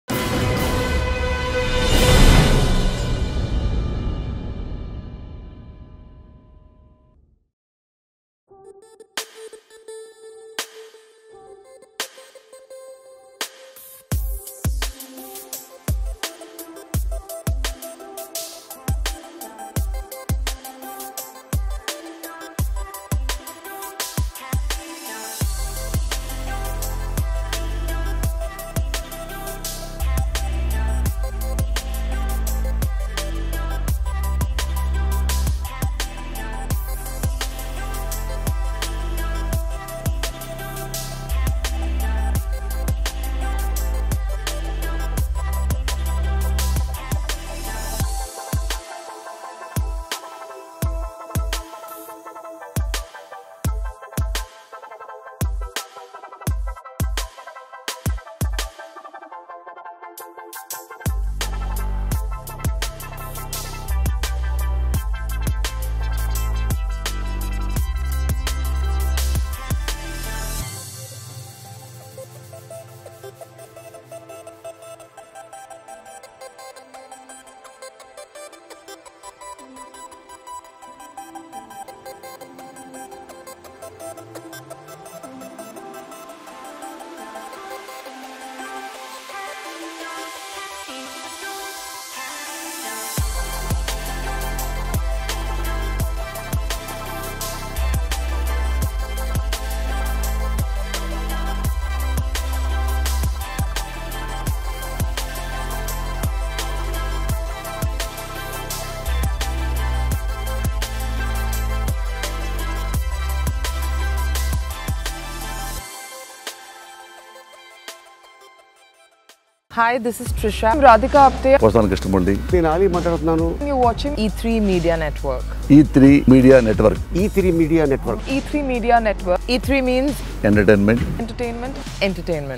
లేడీస్ కి అడ్డొచ్చిన ఆటోడ్రైవర్ ని పక్కకి తోసిన జగనన్న | CM Jagan Angry On Auto Driver | E3 Talkies
Watch లేడీస్ కి అడ్డొచ్చిన ఆటోడ్రైవర్ ని పక్కకి తోసిన జగనన్న | CM Jagan Angry On Auto Driver | E3 Talkies